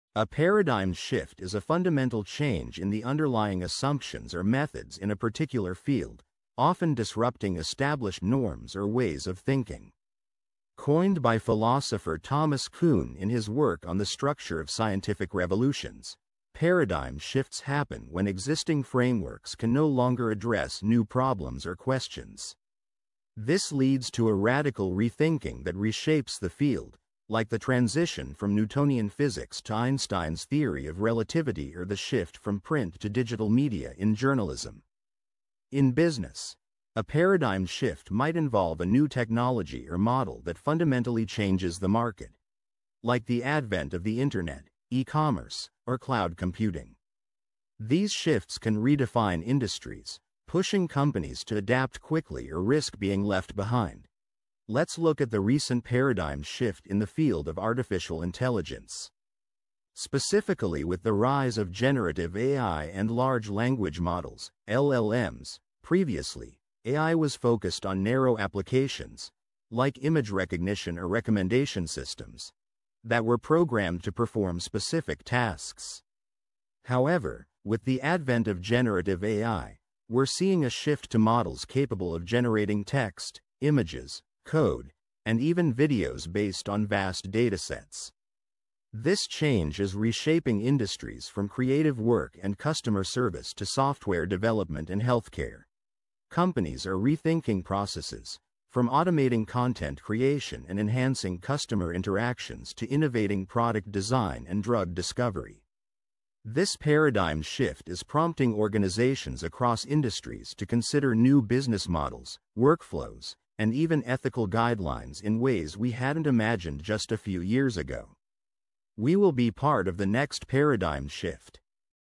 Revolutionary AI Shift: Transforming Our Future! 🤖✨
Join us as we explore the groundbreaking shift in artificial intelligence, showcasing how generative AI is reshaping industries, redefining business models, and revolutionizing workflows. Discover the incredible impact this technology is having on our world today!